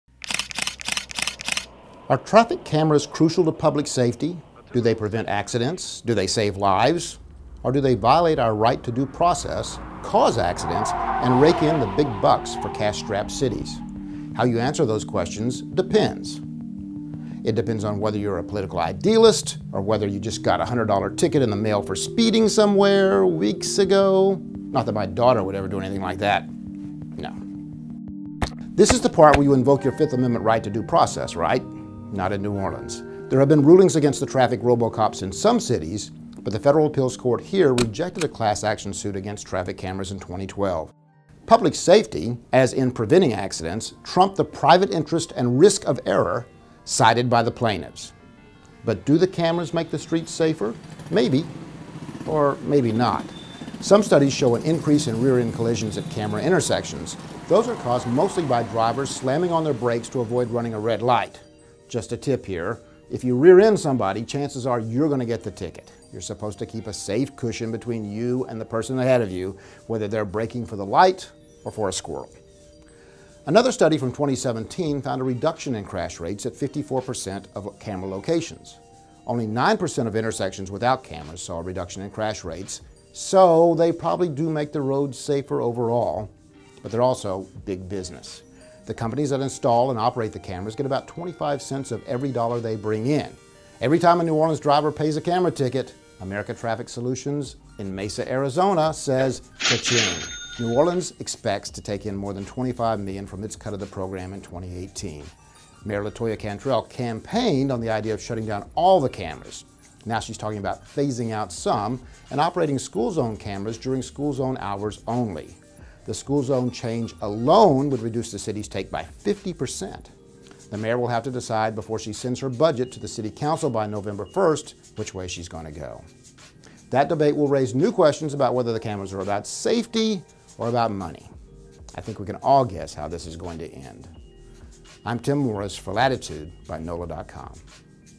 Traffic cameras in New Orleans: Safety vs. Money
Are traffic cameras crucial to public safety? Do they prevent accidents? Or do they violate our right to due process, cause accidents, and rake in the big bucks for cash-strapped cities? Latitude's Tim Morris investigates these questions ahead of New Orleans Mayor LaToya Cantrell's November 1 budget decisions.